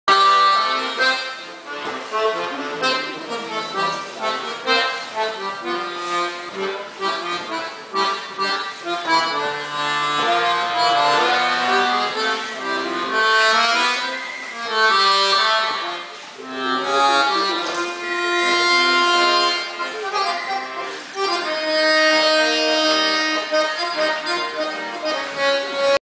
Vertanzt 2011 - Tango-Musik live!
Tango mit Überraschungs-Musik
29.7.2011
Festival Vertanzt
Internationales Festival populärer Tänze
Röthenbach im Emmental